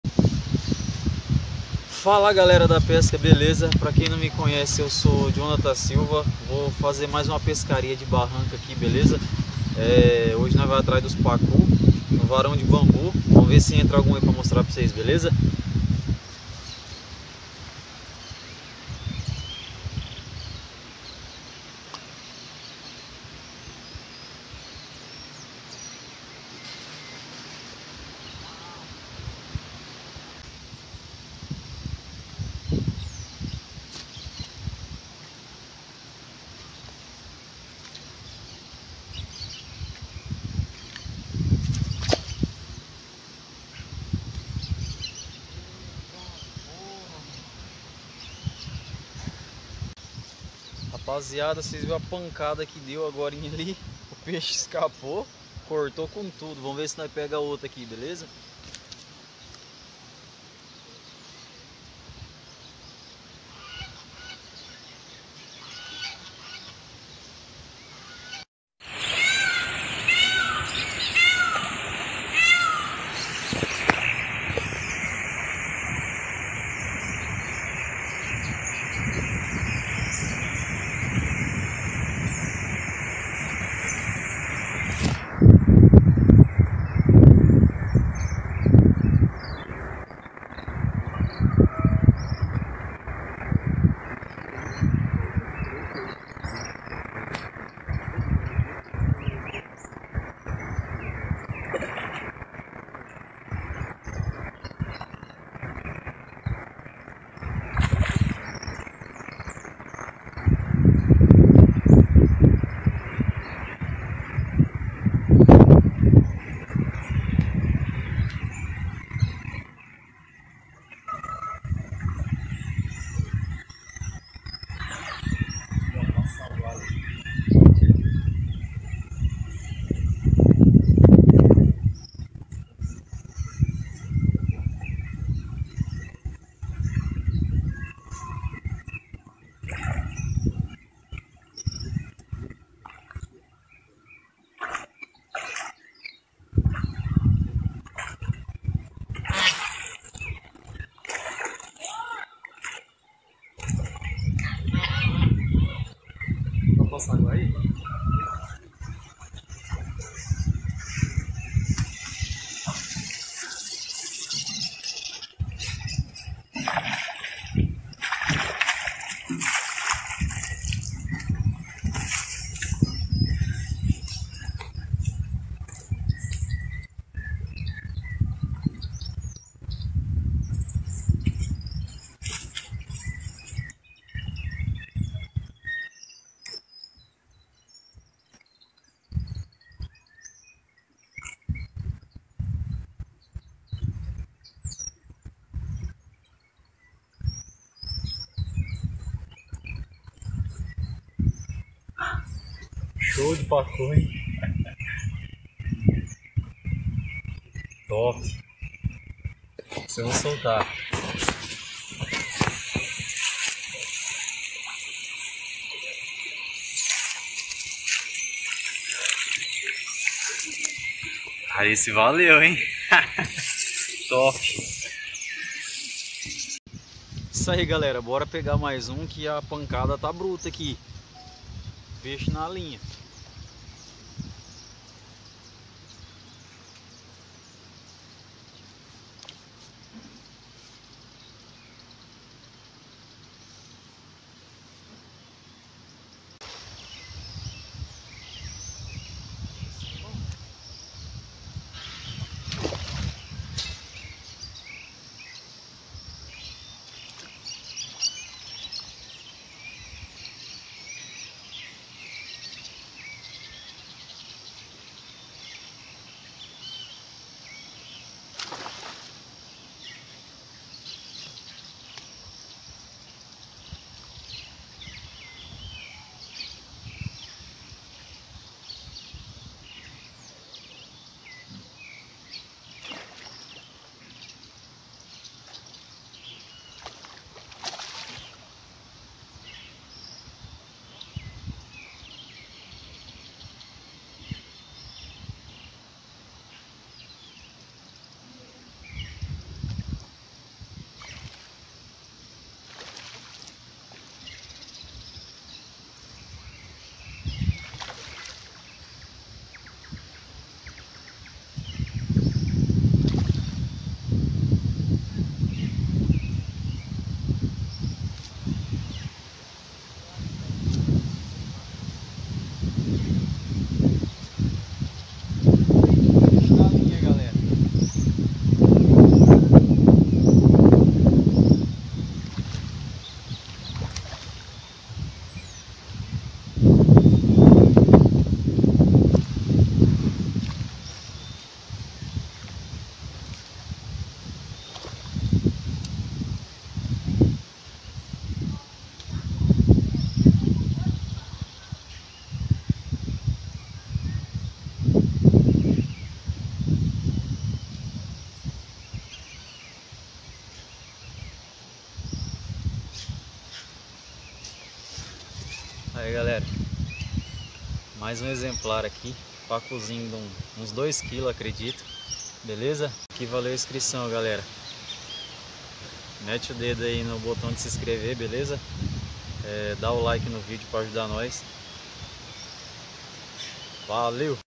Pescaria Bruta de Pacu. Pescaria Raiz ..
Pescaria de Pacu .
Equipamentos usados..
Vara de bambu.
linha 0.60 monofilamento.
chumbo 30 gramas.
anzol maruzeigo n⁰ 18
isca .Goiaba